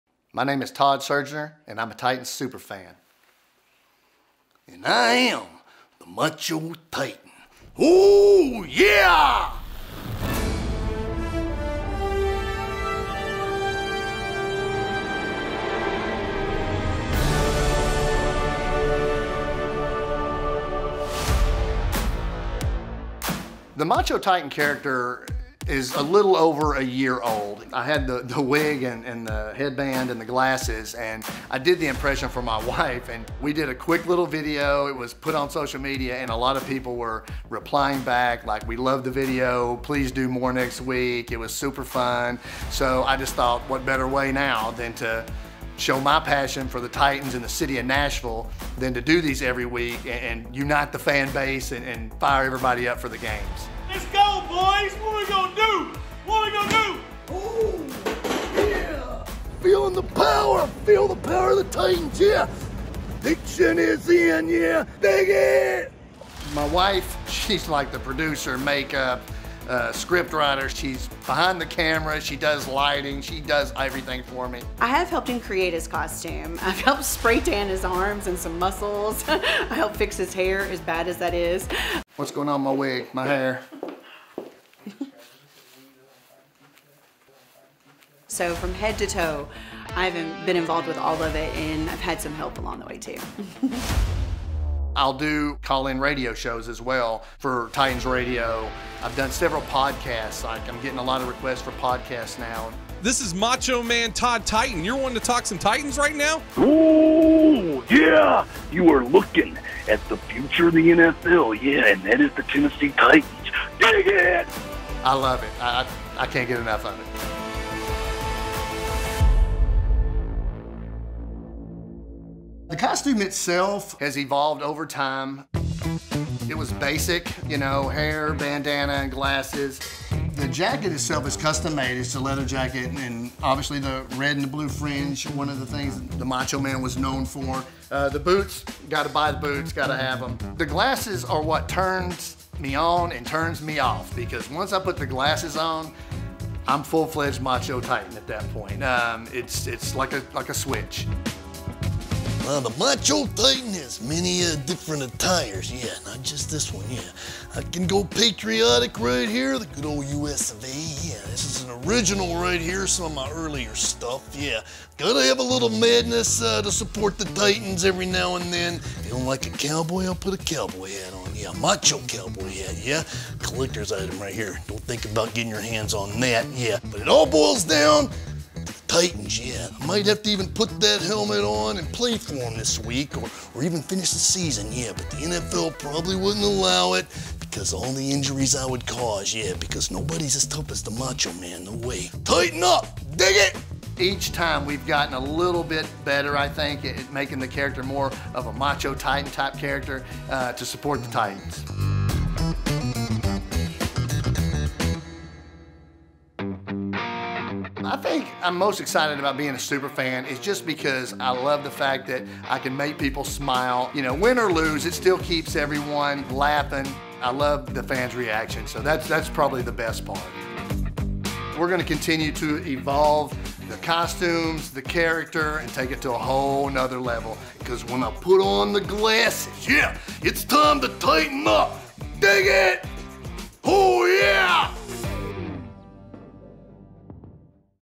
Meet Titans Super Fan "Macho Titan"
Meet Titans Super Fan Todd Surgener, Macho Titan, who's look and hype videos have evolved since reaching many Titans fans on social media. Macho Titan always shares his excitement for the Titans and the Nashville community.